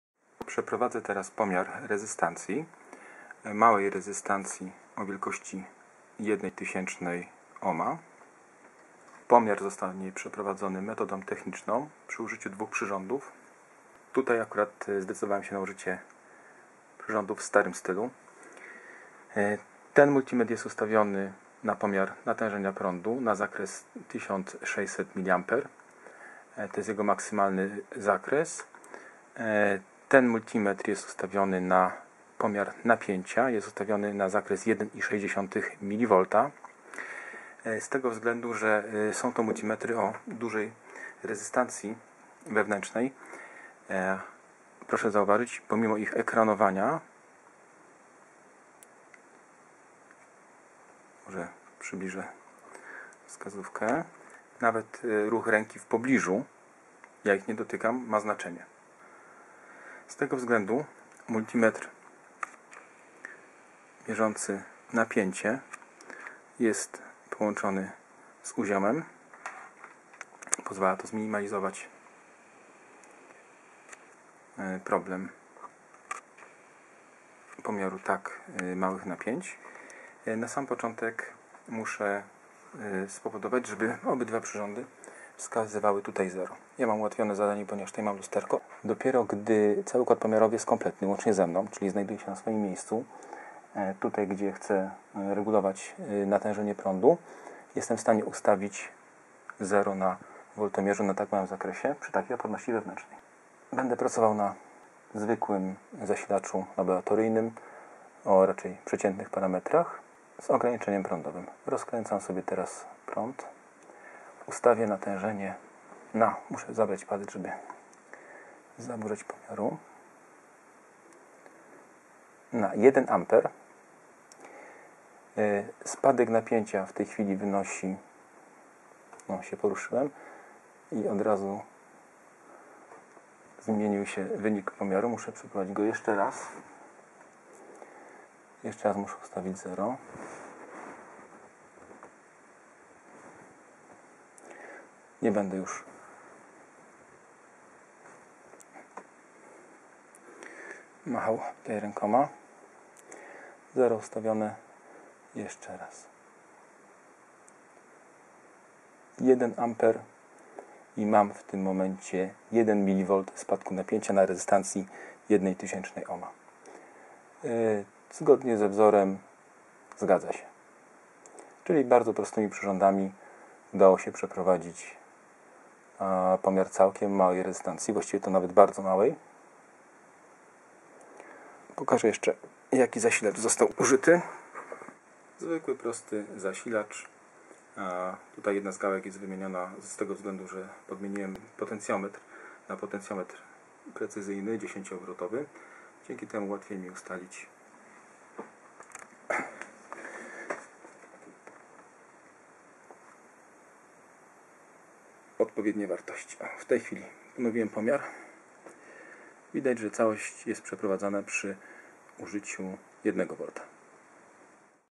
Pomiar rezystancji metodą techniczną - prąd DC, multimetr analogowy
Użyte przyrządy pomiarowe:
Użyte przyrządy pomiarowe:
- multimetr: CHAUVIN ARNOUX GI83
- rezystor wzorcowy
- zasilacz: DF-1731SBC-3A